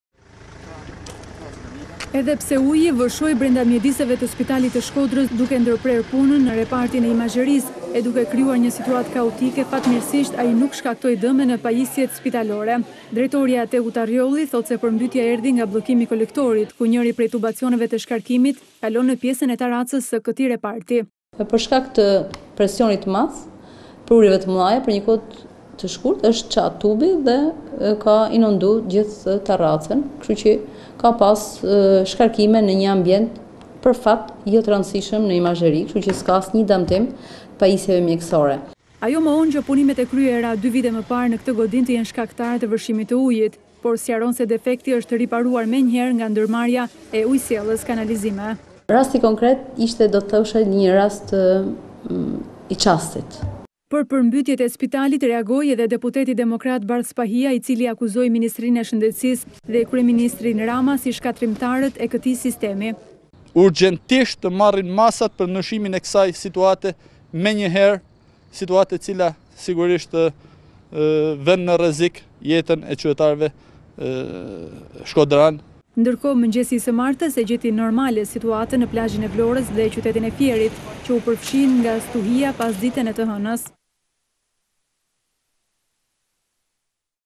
Ora News - Bllokimi i kolektorit përmbyti spitalin e Shkodrës, shpëtojnë pajisjet spitalore
Edhe pse uji vërshoi brenda mjediseve të spitalit të Shkodres duke ndërprerë punën në repartin e imazherisë, e duke krijuar një situatë kaotike, fatmirësisht ai nuk shkaktoi dëme në pajisjet spitalore.

Lajmi konfirmohet nga drejtoresha Teuta Rrjolli, e cila deklaron se është zbuluar edhe shkaku i përmbytjes, që sipas saj ka ardhur nga bllokimi i kolektorit, ku njëri prej tubacioneve të shkarkimit kalon në pjesën e tarracës së këtij reparti.

"Presioni i madh i ujit çau tubin në tarracë. Nuk ka asnjë dëmtim në pajisje mjekësore."

Ajo mohon që punimet e kryera dy vite më parë në këtë godinë të jenë shkaktare të vërshimit të ujit, por sqaron se defekti është riparuar menjëherë nga ndërmarrja e ujësjellës–kanalizime.

Për përmbytjet e spitalit reagoi edhe deputeti demokrat Bardh Spahia që akuzoi Ministrinë e Shëndetësisë dhe kryeministrin Rama, si shkatërrimtarët e këtij sistemi.

Bardh Spahia: “Mungesa e vëmendjes së qeverisë Rama ndaj mjekësisë dhe Spitalit Rajonal të Shkodrës solli që mjekët dhe stafi mjekësor i këtij spitali të jenë në mëshirë të fatit. Në pak minuta shi u përmbyt i gjithë shërbimi i radiologjisë dhe jo vetëm. 2 vite më parë ka pasur një rikonstruksion të kësaj godine e cila e bën edhe me shqetësuese dhe implikon institucionet drejtuese të Shëndetësisë shqiptare, që duket se nuk kanë aspak prioritet shëndetin e qytetarëve por kanë interes mbushjen e xhepave të tyre dhe të atyre oligarkëve pranë tyre. Me keqardhje e them që është një degradim i përgjithshëm i sistemin shëndetësor shqiptar dhe strukturave të këtij sistemi dhe falë kësaj po mbetet degradimi i spitalit të Shkodrës”.